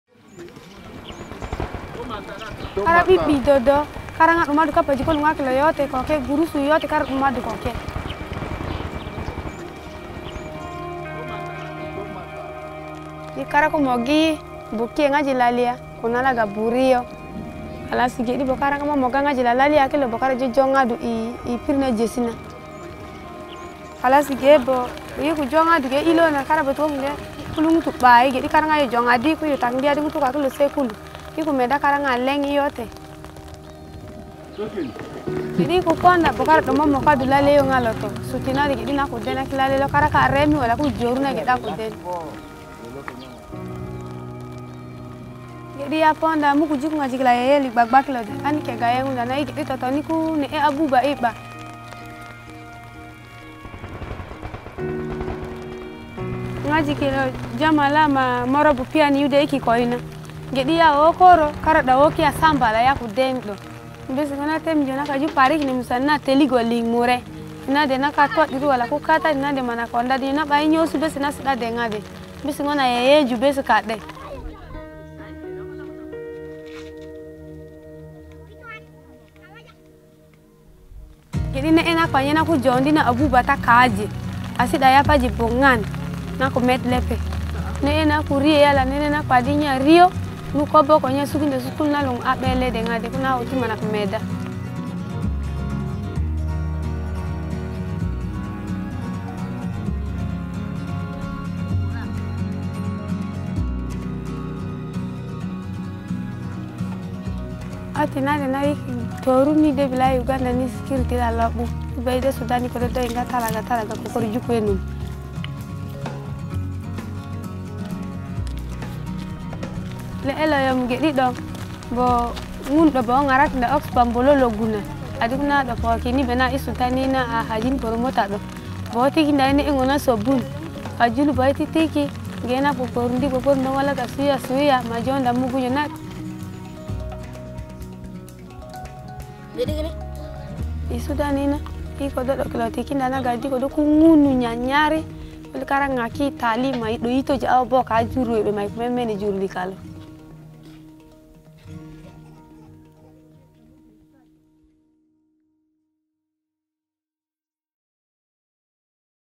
Amina Running From War | Oxfam GB
One million people have fled the brutal conflict in South Sudan and taken refuge in neighbouring Uganda thanks to a generous refugee policy by the country’s government.
Amina fled South Sudan with her children and is now living in a refugee settlement in Uganda. Oxfam is there helping thousands of people like Amina.